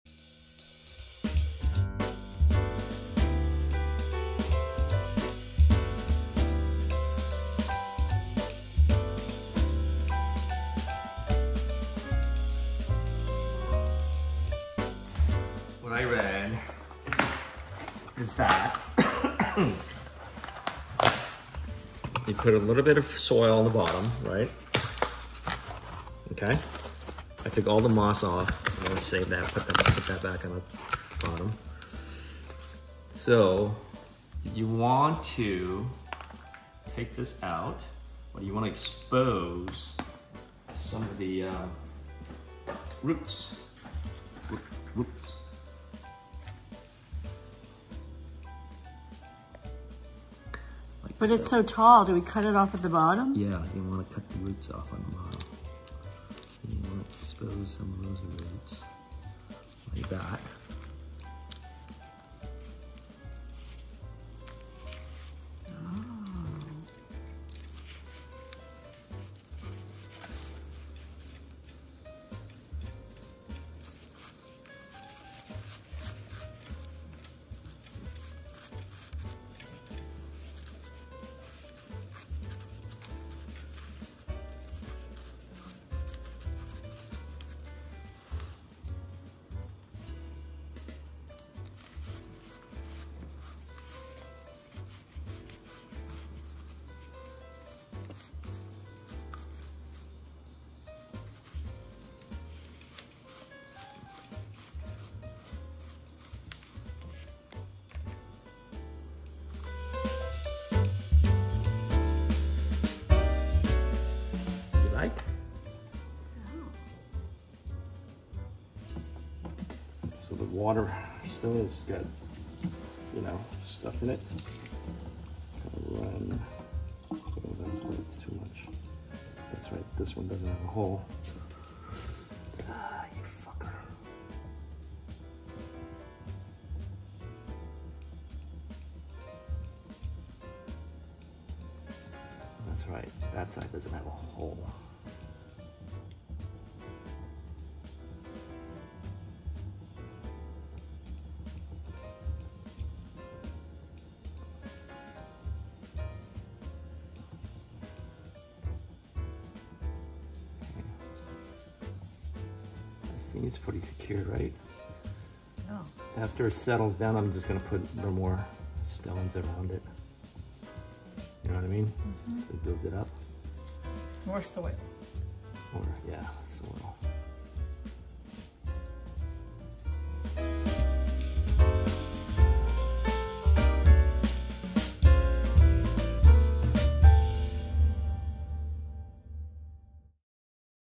My husband repots the bonsai
My husband repots the bonsai. The original one was a small pine tree that my sister gave me about five years ago. I kept it alive until last year. Today, we are going to find a bonsai to replant into the original pot (which had a little accident when the Bengos visited us for Thanksgiving and were playing ping pong using a skillet lid for a ping pong paddle. Long story short, the bonsai saved the kitchen window from smashing but sacrificed itself). The pot was repaired but the pine tree was never the same. It died the next year (last year). Now, for my birthday, I requested this outing to replant the original broken-but-repaired pot. Very wabi-sabi.